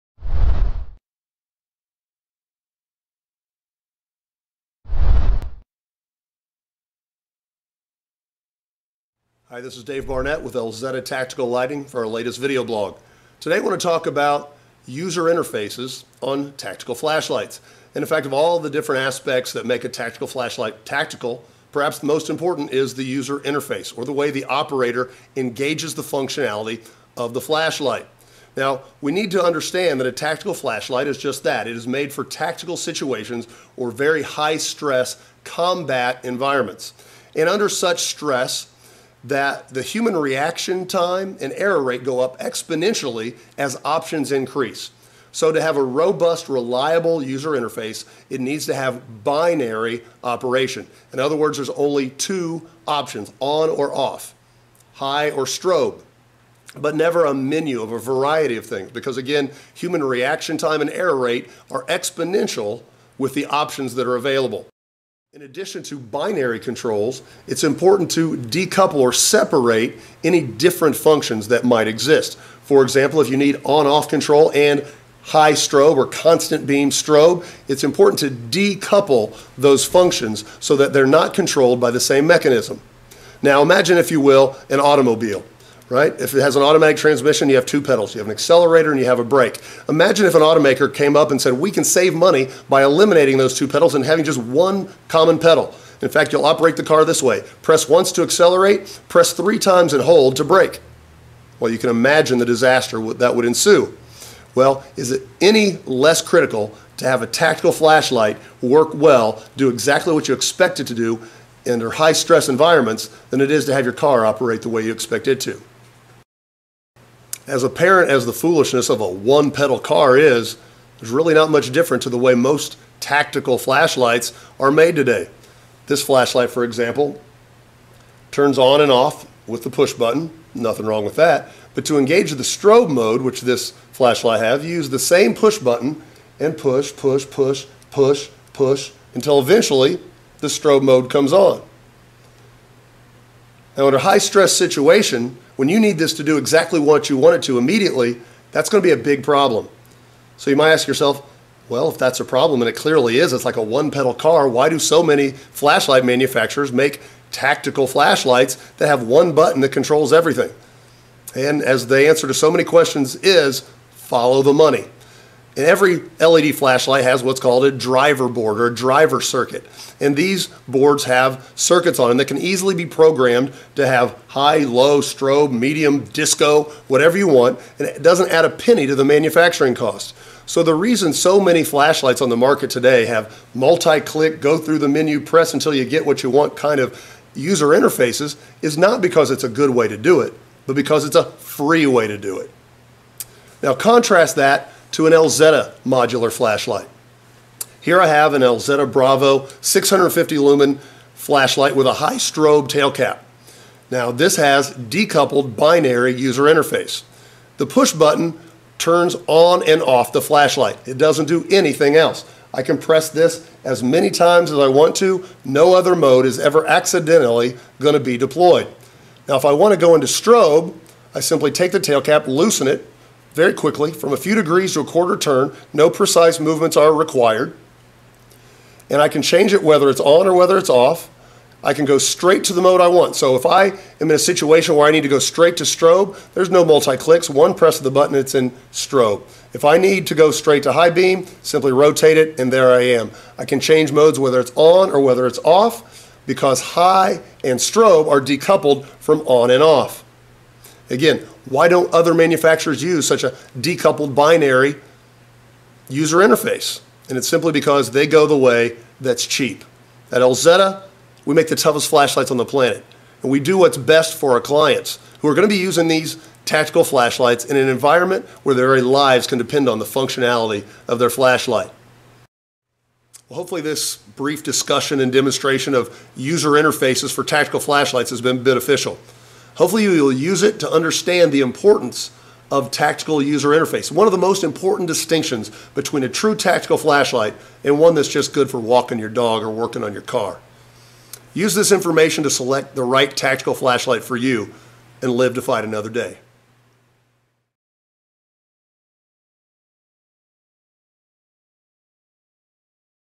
User Interface for Tactical Flashlights, Decoupled Binary Controls, Elzetta vlog episode 4
One of the most critical characteristics that separates a true Tactical Flashlight from the non-tactical variety is the User Interface.  While many LED flashlights on the market today claim to be "tactical", their method of operation is wholly unsuited to tactical applications.  A flashlight with a multi-click user interface should never be used in high stress combat environments.  Watch this brief video and learn why truly tactical devices need decoupled binary controls.  Your life may depend on it.
Elzetta manufactures The Toughest Flashlights on the Planet; fully modular and Made in USA.